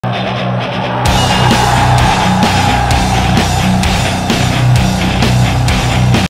Dinamo revine in Liga 1 !
Dinamo revine in liga 1 dupa un sezon petrecut in liga inferioara